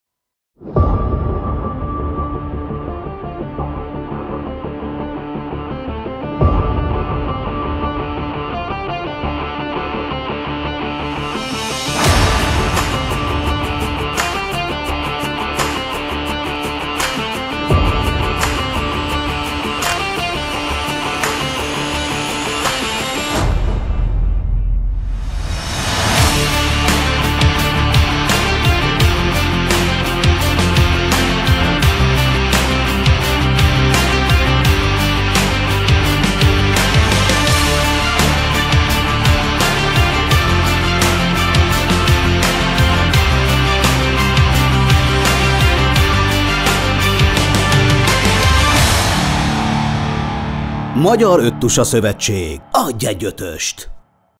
Hamarosan Absolutio - Búcsú a lovaktól színházi előadás
Alig öt óra múlva, rögtön az öttusa Európa-bajnokság mix váltóinak versenye után várunk mindenkit szeretettel ingyenes színházi előadásunkra, mellyel igyekszünk méltó búcsút venni évszázados társainktól, a lovaktól.

Az előadás fő vonala a Coubertin-legenda lesz, mely arról a katonáról szól, akinek lovagolva, lőve, vívva, úszva és futva kell célbaérnie egy fontos üzenettel.

A helyszín pedig nem is lehet más, mint a budapesti Európa-bajnokság szíve, a Puskás Aréna Szoborparkja!

Absolutio - Búcsú a lovaktól, július 14., 18:00.